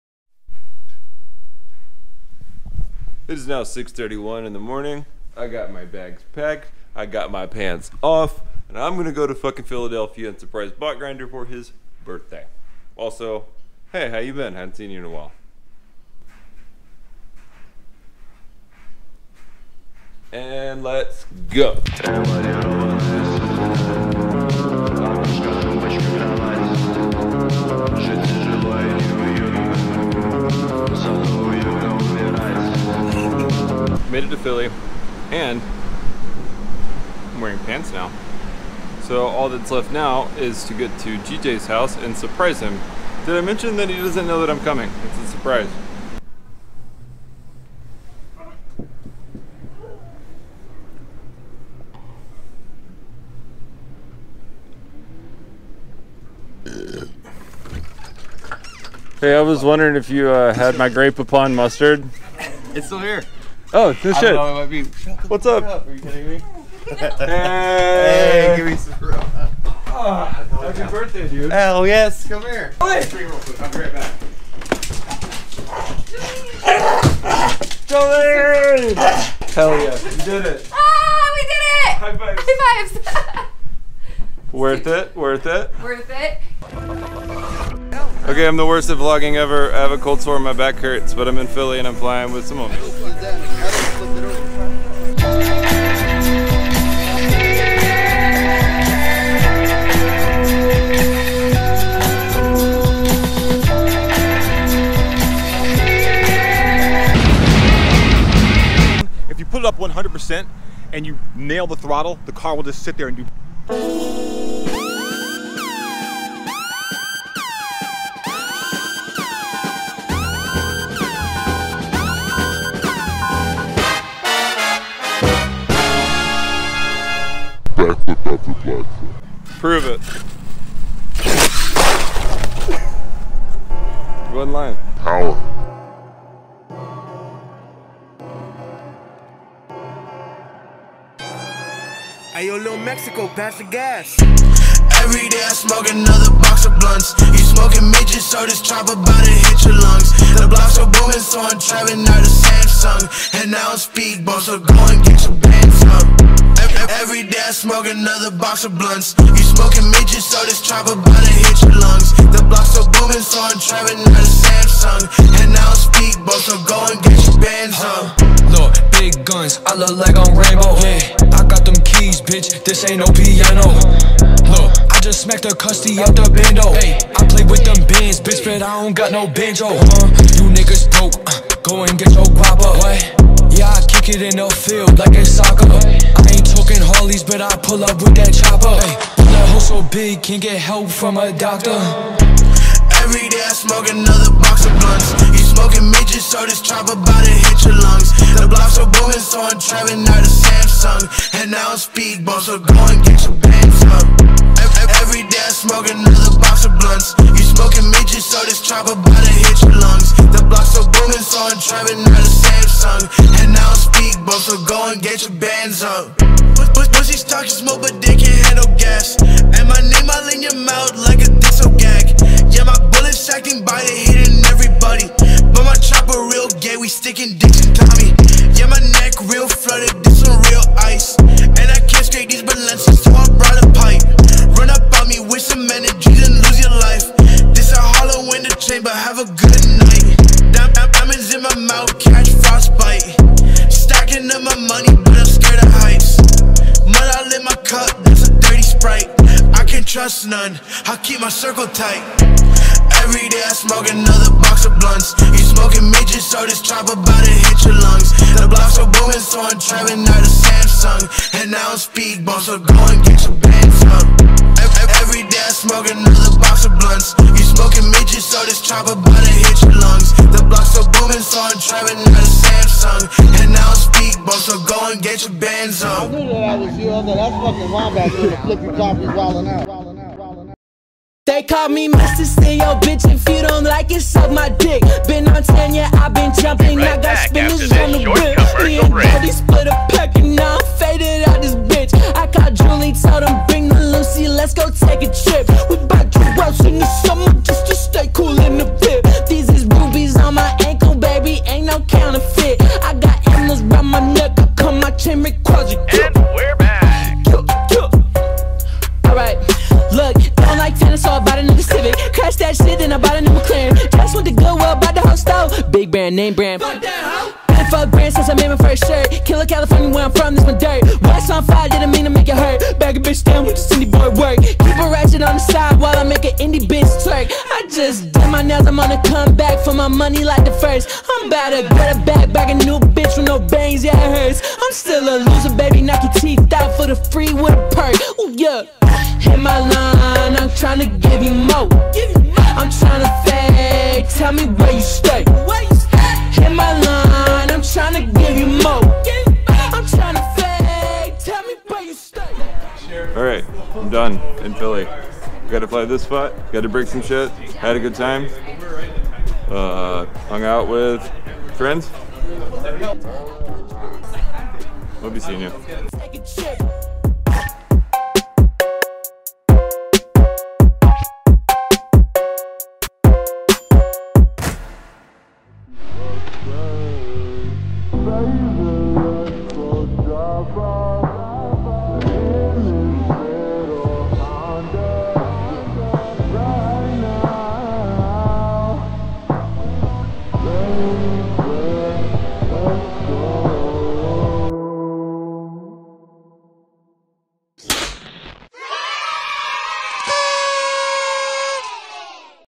Bots Birthday Surprise
HUUUGE shoutout to Ashley for makin this happen.  THANK YOU SO MUCH.
Also, im bad at vlogging so I had to edit the fuck out of what I did have.  Only had two usable flight clips from the weekend but i had a fucking blast.

Thank you PHILLY for staying rad af.
-much love

BQE.io for all of your mulitcopter needs.

DONT SUCK!